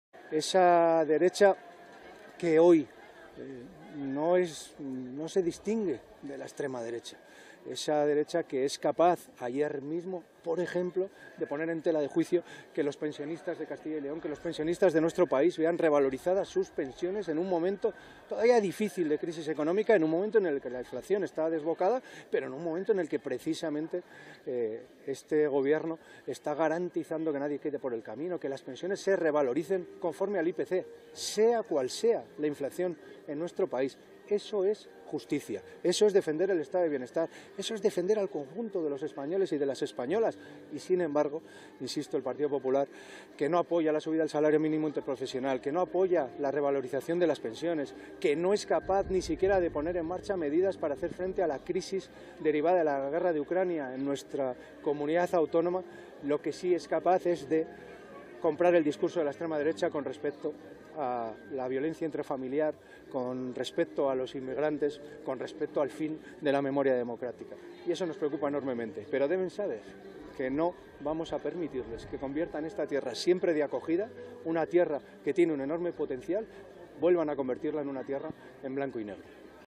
23/04/2022 El PP pone en tela de juicio que los pensionistas de CyL vean revalorizadas sus pensiones
El PP pone en tela de juicio que los pensionistas de Castilla y León vean revalorizadas sus pensiones.

Feijóo quiere recortar las pensiones.

El Gobierno de España garantiza que nadie quede atrás y se revaloricen con el IPC.